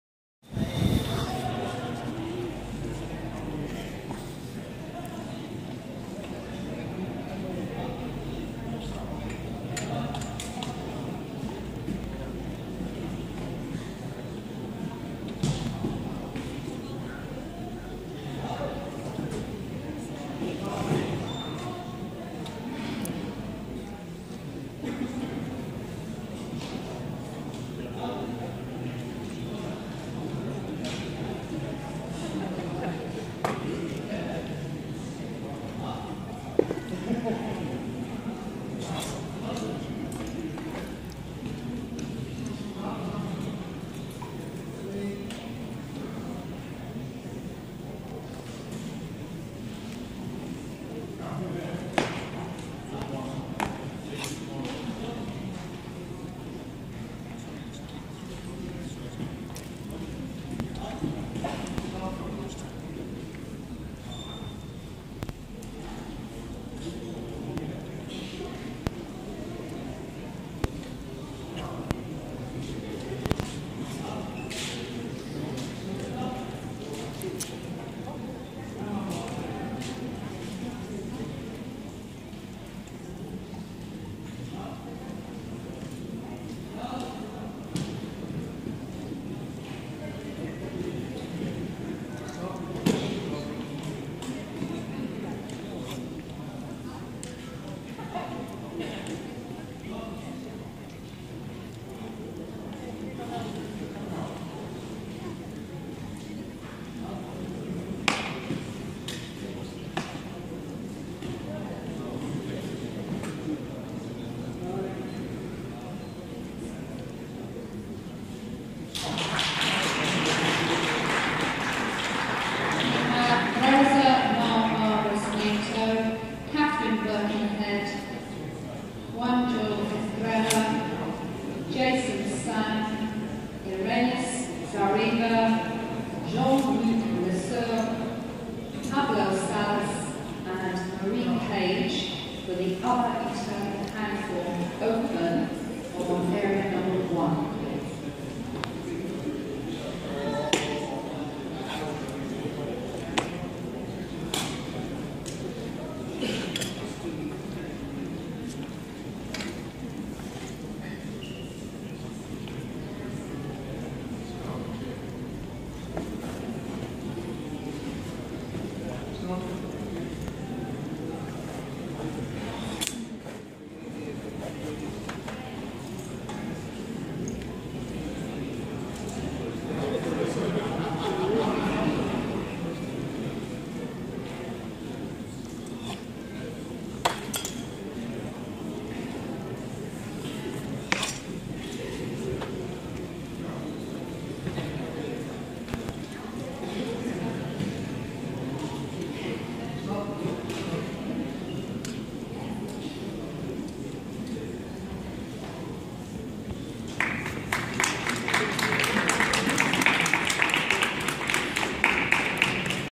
Chen Taiji Jian Form - 7 TFCE European Championships, Oxford 2015
Chen Taiji Jian Form at the VII TFCE European Championships. Oxford, 2015